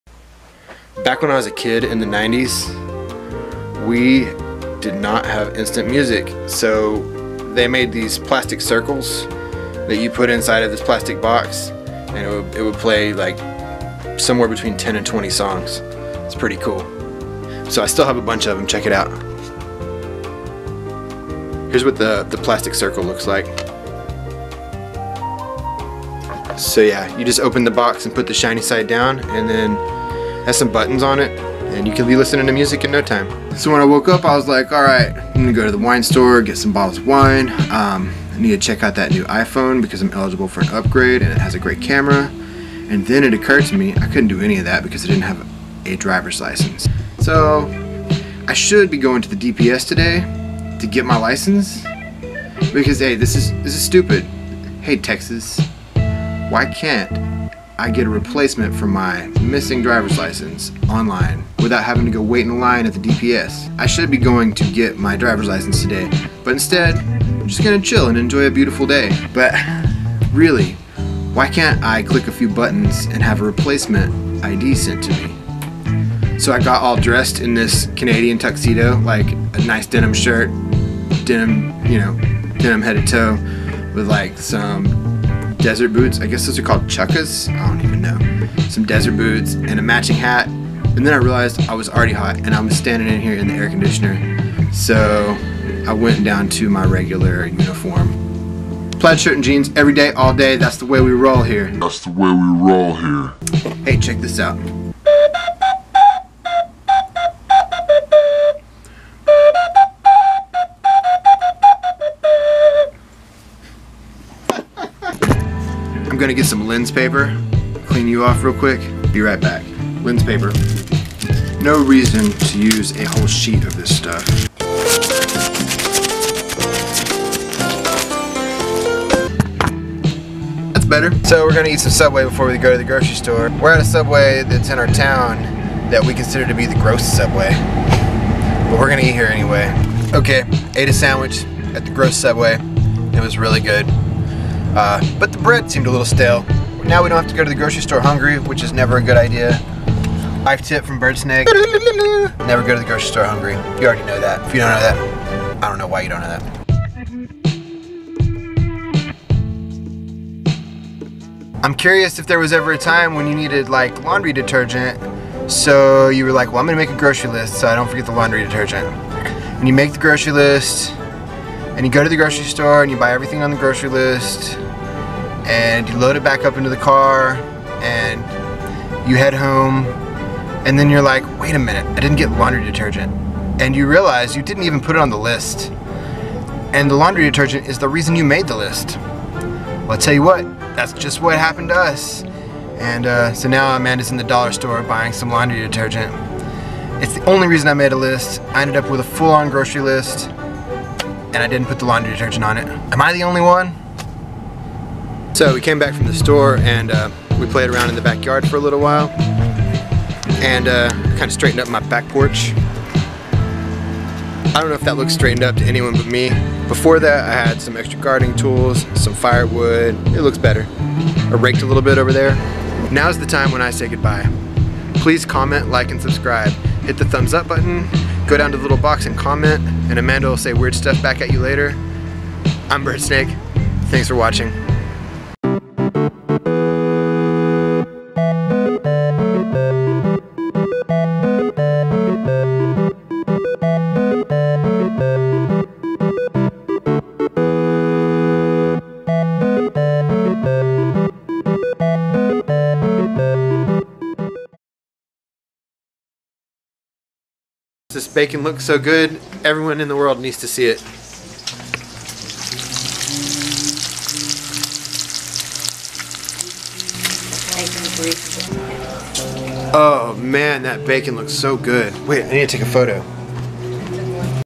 How CDs Work, No Drivers License, and Gross Subway
I explain how music CDs work. I play the mermaid flute. We eat at gross Subway and go to the grocery store. It's a good day.